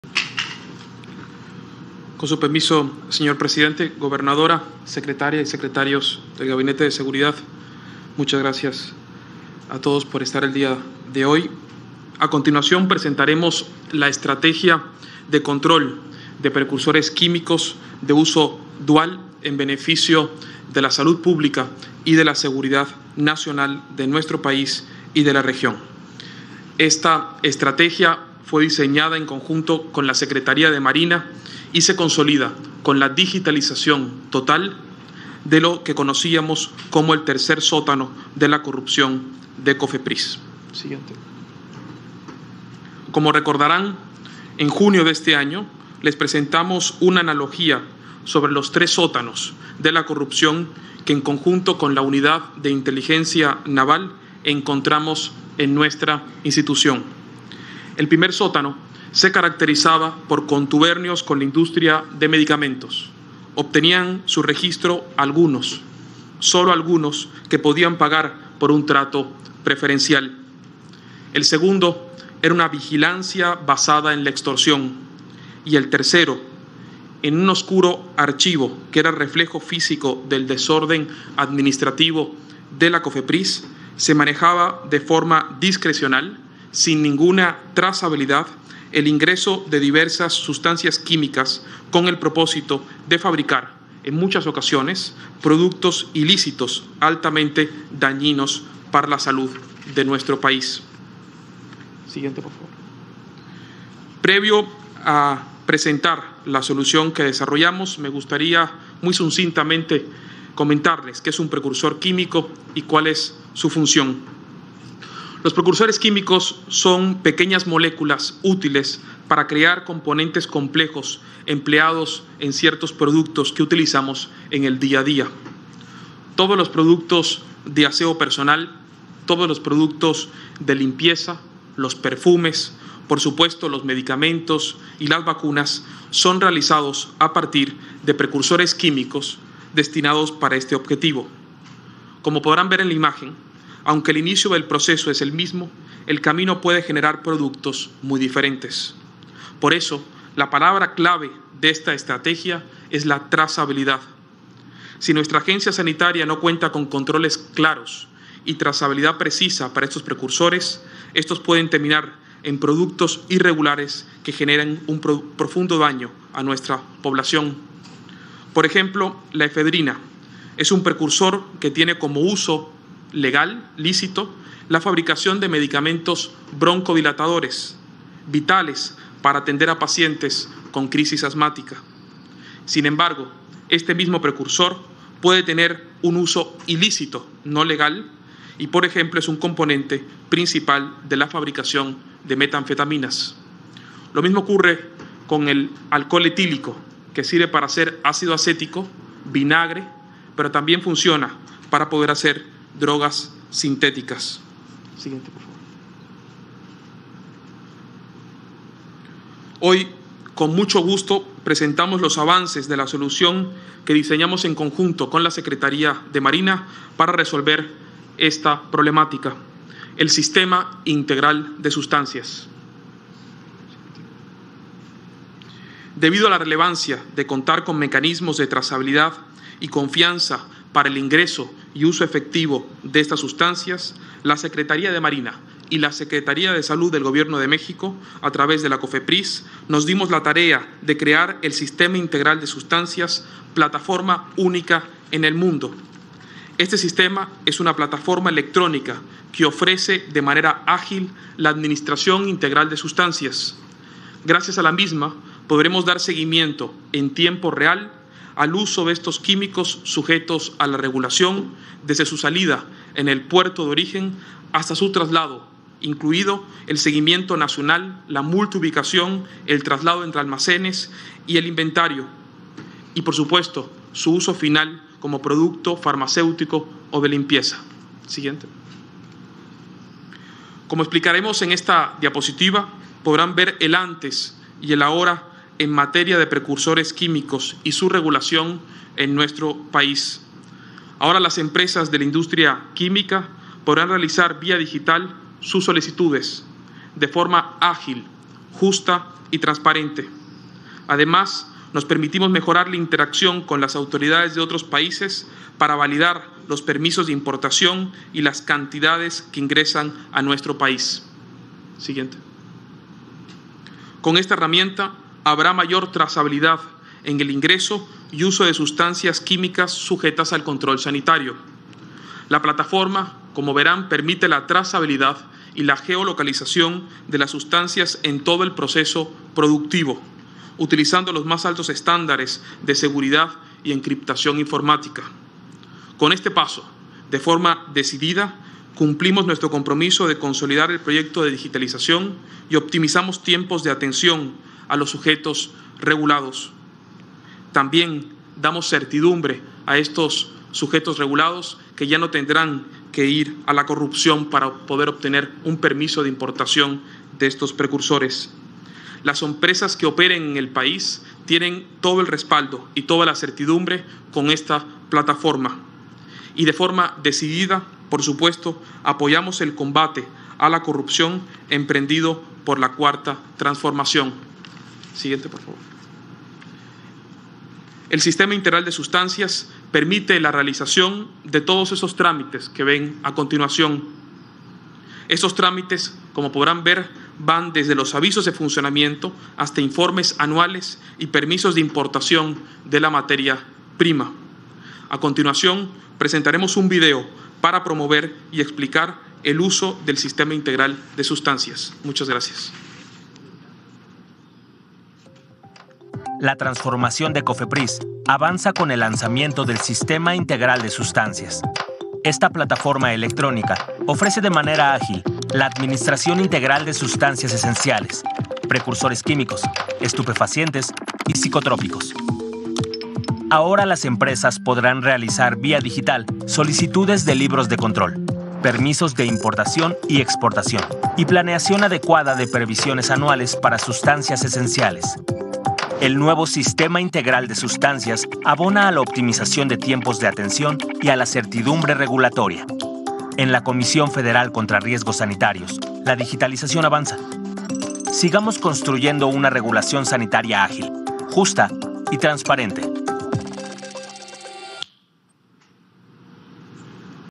Cofepris y Secretaría de Marina regulan sustancias controladas de forma ágil y transparente
a Comisión Federal para la Protección contra Riesgos Sanitarios (Cofepris) y la Secretaría de Marina (Semar) presentaron hoy, durante la conferencia matutina encabezada por el presidente de la República, Andres Manuel López Obrador, la plataforma digital Sistema Integral de Sustancias.